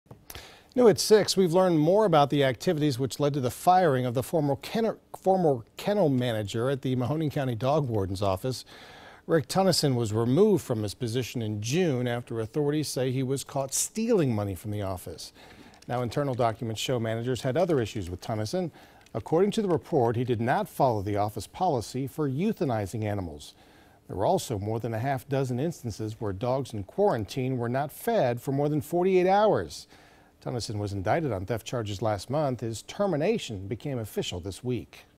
Mahoning Co. Dog Warden: Kennel manager improperly euthanized dogs
Documents detail troubling accusations against the former kennel manager of the Mahoning County Dog Warden's Office.